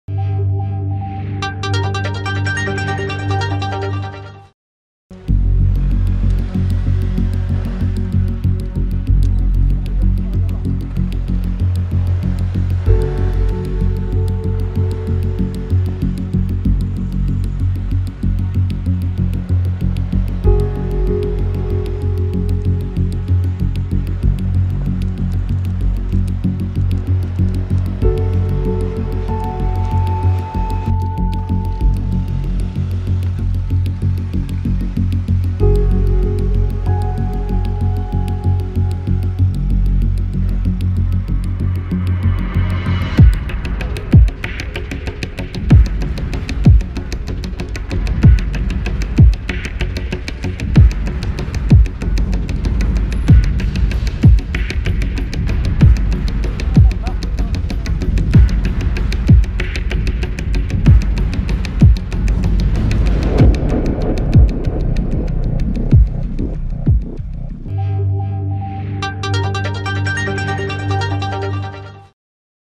مشاو فيها ..اعتقال شواذ لابسين ملابس نسائية مبحوث عنهم وسط كازا
عاينت شوف تيفي اعتقال شواء بمدينة الدار البيضاء خلال فترة الليل